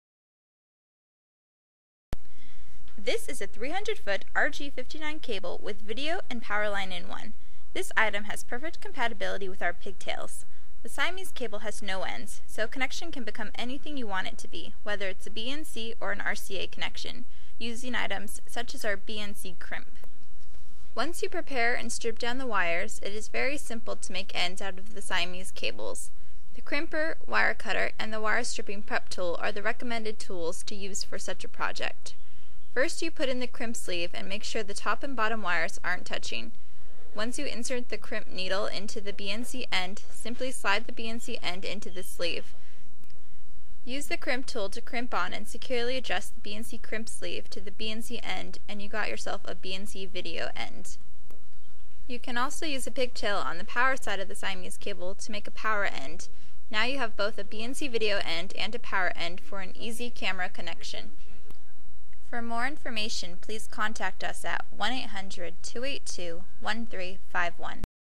Asianwolf 300 Foot RG59 Siamese Cable Video and Power Line from asianwolf.com
This is a 300 Foot RG59 CABLE WITH VIDEO AND POWER LINE IN ONE. This item has perfect compatibility with our pigtails.  The Siamese cable has no ends, so connection can become anything you want it to be, whether it's a BNC or an RCA connection, using items, such as, our BNC Crimp.
 Once you prepare and strip down the wires it is very simple to make ends out of the Siamese cables.  The crimper, wire cutter, and the wire stripping prep tool are the recommended tools to use for such a project.  First you put in the crimp sleeve and make sure the top and bottom wires aren't touching.  Once you insert the crimp needle in to the bnc end simply slide the bnc end into the sleeve.  Use the crimp tool to crimp on and securely adjust the bnc crimp sleeve to the bnc end and you got yourself a bnc video end.  You can also use a pigtail on the power side of the Siamese cable to make a power end.  Now you have both a bnc video end and a power end for an easy camera connection.